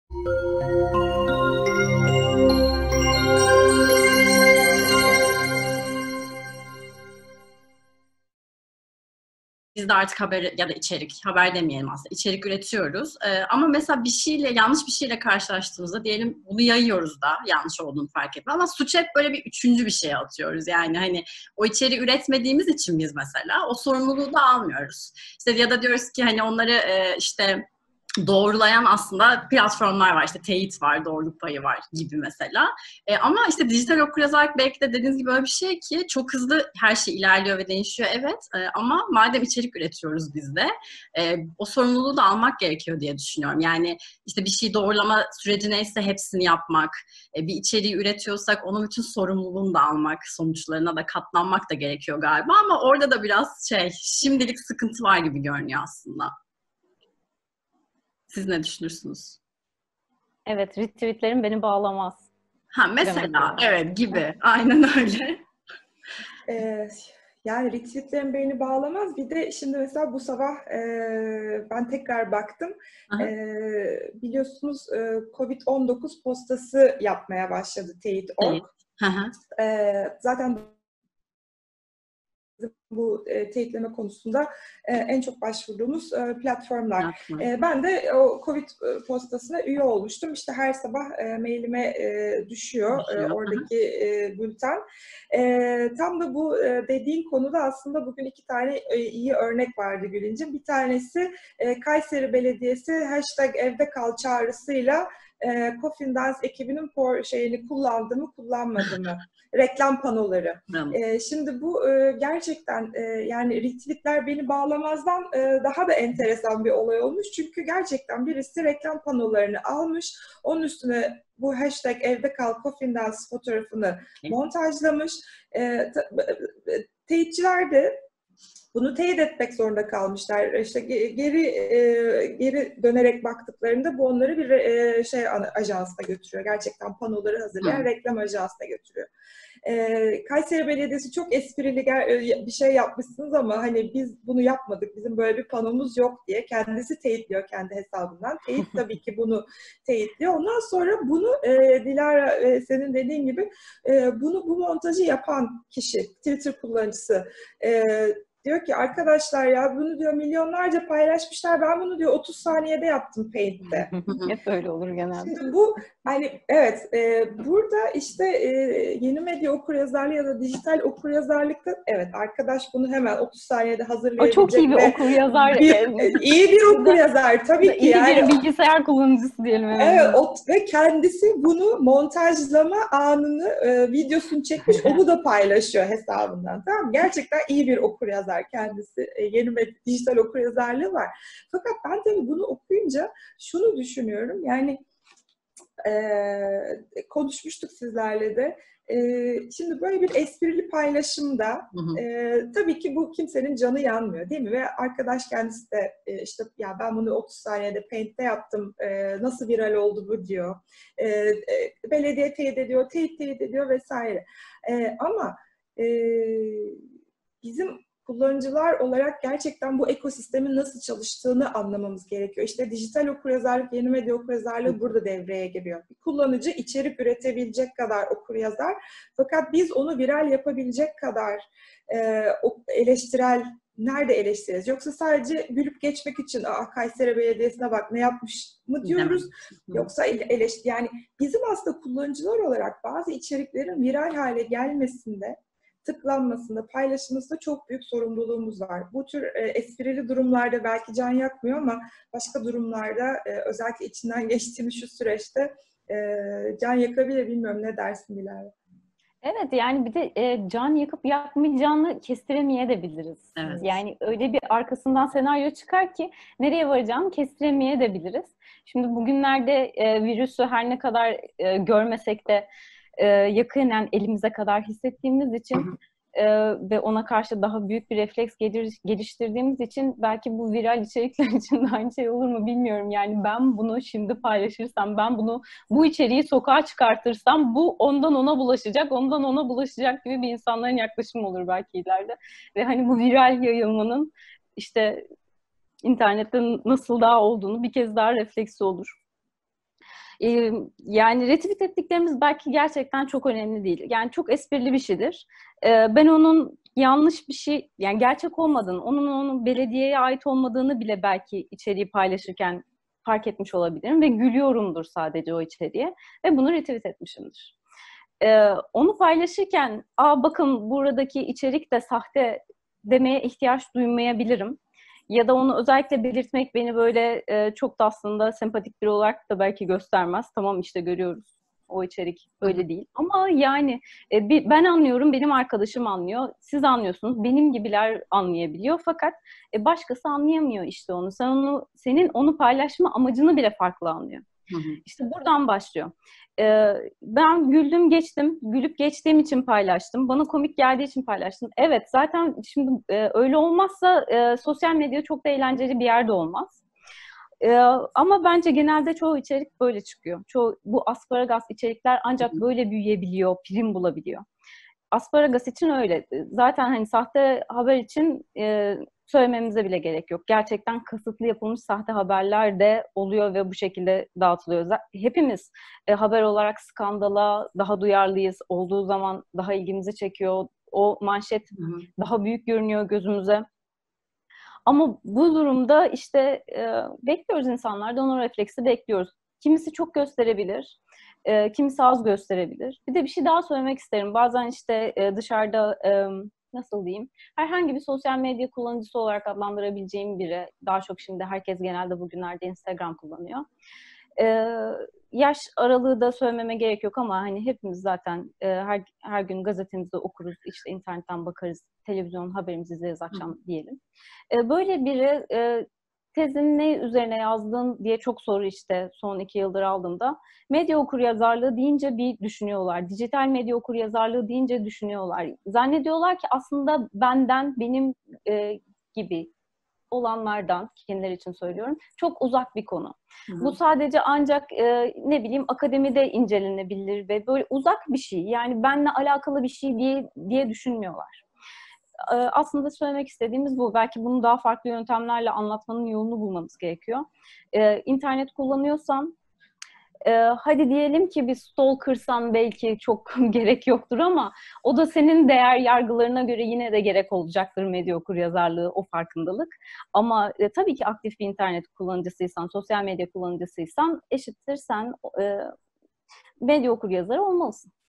#3 Dijital Okuryazarlık ve İçerik Üretimi
Kadir Has Üniversitesi Yeni Medya Bölüm Başkanı Doç. Dr. Eylem Yanardağoğlu ve Yeni Medya Uzmanı Dilara Eldaş Baş anlatıyor.